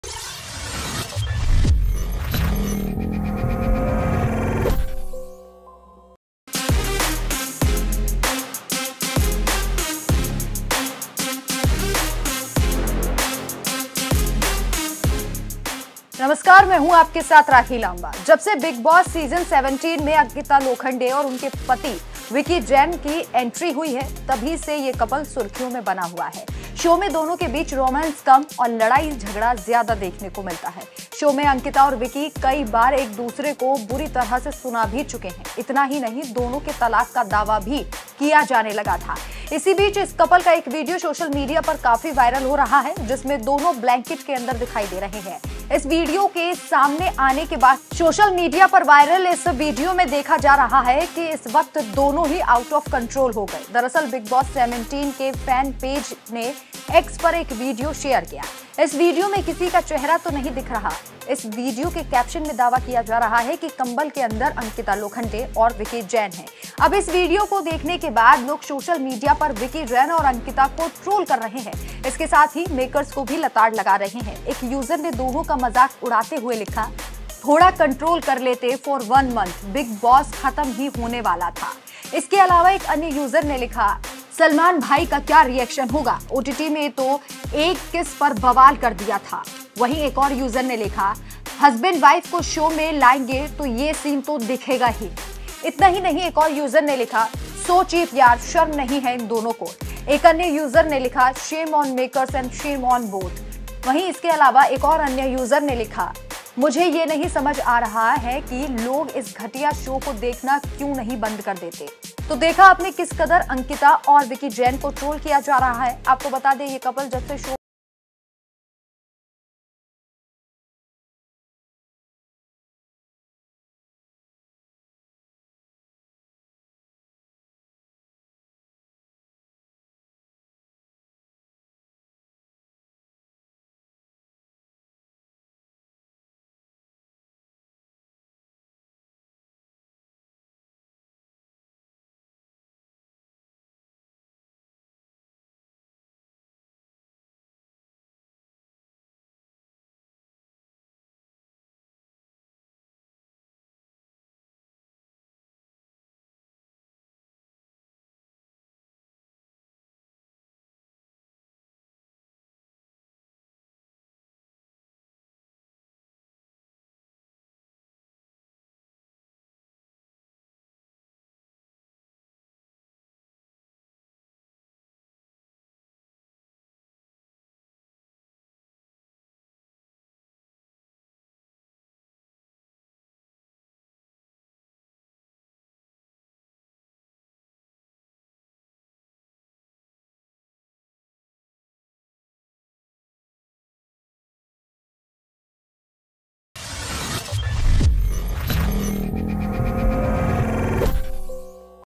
Ankita Lokhande-Vicky ने जमाने के सामने कर डाली ऐसी हरकत, लोगों ने सुनाई खरी -खरी !
जबसे बिग बॉस सीजन 17 में अंकिता लोखंडे और उनके पति विक्की कोशल की एंट्री हुई है, तभी से ये कपल सुर्खियों में बना हुआ है, शो में दोनों के बीच रोमांस कम और लड़ाई झगड़ा ज्यादा देखने को मिलता है…शो में अंकिता और विक्की कई बार एक दूसरे को बुरी तरह सुना चुके हैं,इतना ही नहीं दोनों के तलाक का दावा तक किया जाने लगा है…
Thumb01: अंकिता - विक्की ने नेशनल टीवी पर की ऐसी हरकत !
   Chandrakala
 prashant.